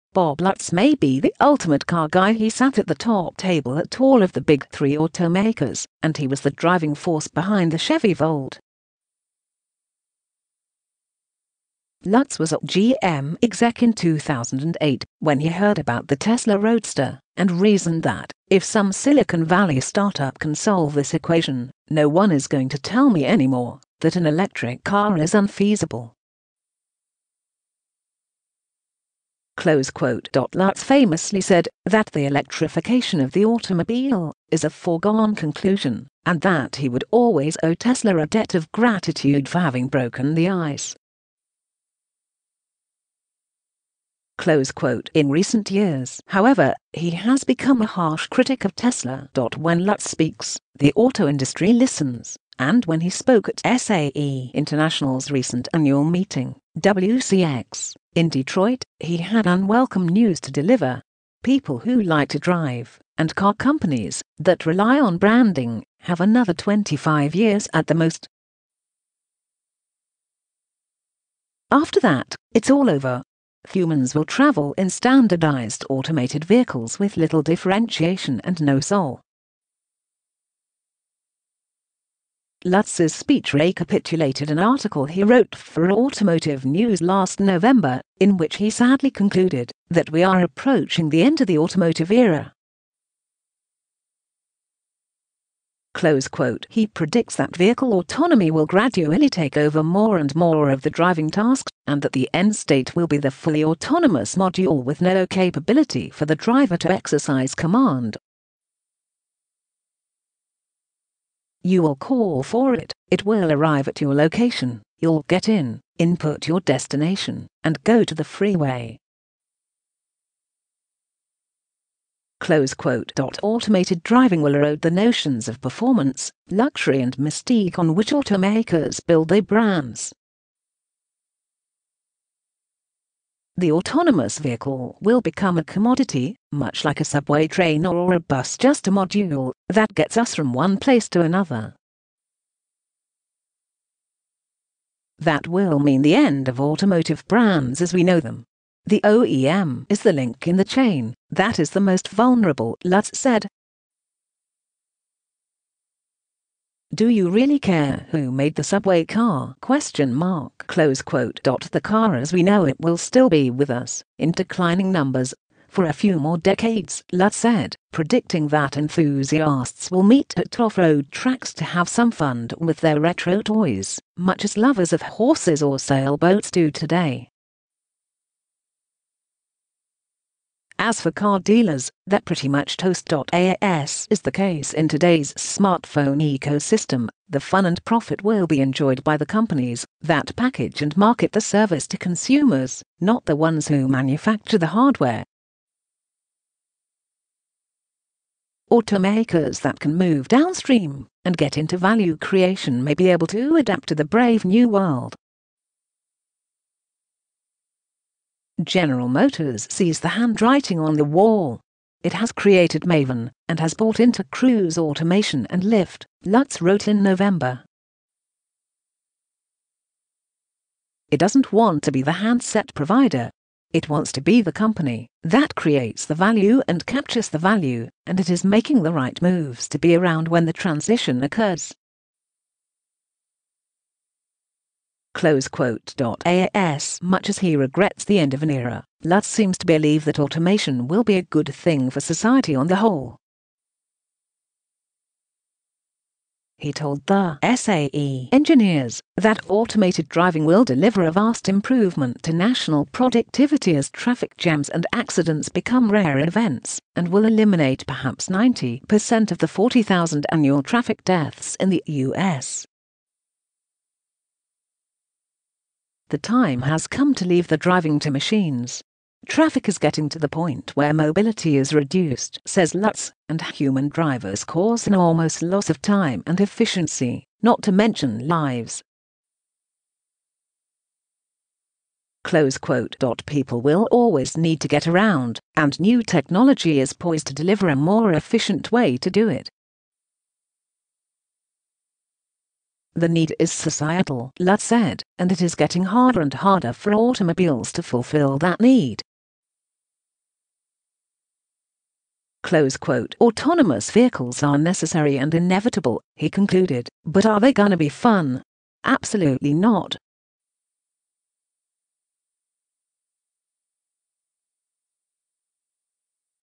BOB LUTZ: IT’S ALL OVER FOR AUTO BRANDS AS AUTONOMOUS VEHICLES WILL CHANGE EVERYTHING
Bob Lutz may be the ultimate car guy - he sat at the top table at all of the Big Three automakers, and he was the driving force behind the Chevy Volt. Lutz was a GM exec in 2008 when he heard about the Tesla Roadster, and reasoned that “if some Silicon Valley startup can solve this equation, no one is going to tell me anymore that [an electric car] is unfeasible.”

Lutz famously said that “the electrification of the automobile is a foregone conclusion,” and that he would always owe Tesla a debt of gratitude for having “broken the ice.” In recent years, however, he has become a harsh critic of Tesla.

When Lutz speaks, the auto industry listens, and when he spoke at SAE International’s recent annual meeting (WCX) in Detroit, he had unwelcome news to deliver: people who like to drive, and car companies that rely on branding, have another 25 years at the most. After that, “it’s all over.” Humans will travel in standardized automated vehicles with little differentiation and no soul.

Lutz’s speech recapitulated an article he wrote for Automotive News last November, in which he sadly concluded that “we are approaching the end of the automotive era.” He predicts that vehicle autonomy will gradually take over more and more of the driving task, and that “the end state will be the fully autonomous module with no capability for the driver to exercise command. You will call for it, it will arrive at your location, you’ll get in, input your destination and go to the freeway.”

Automated driving will erode the notions of performance, luxury and mystique on which automakers build their brands. The autonomous vehicle will become a commodity, much like a subway train or a bus - just a module that gets us from one place to another. That will mean the end of automotive brands as we know them. “The OEM is the link in the chain that is the most vulnerable,” Lutz said. “Do you really care who made the subway car?”
“The car as we know it will still be with us, in declining numbers, for a few more decades,” Lutz said, predicting that enthusiasts will meet at off-road tracks to have some fun with their retro toys, much as lovers of horses or sailboats do today. As for car dealers, they’re pretty much toast.

As is the case in today’s smartphone ecosystem, the fun and profit will be enjoyed by the companies that package and market the service to consumers, not the ones who manufacture the hardware. Automakers that can “move downstream and get into value creation” may be able to adapt to the brave new world. “General Motors sees the handwriting on the wall. It has created Maven and has bought into Cruise Automation and Lyft,” Lutz wrote in November. “It doesn’t want to be the handset provider. It wants to be the company that creates the value and captures the value, and it is making the right moves to be around when the transition occurs.”

As much as he regrets the end of an era, Lutz seems to believe that automation will be a good thing for society on the whole. He told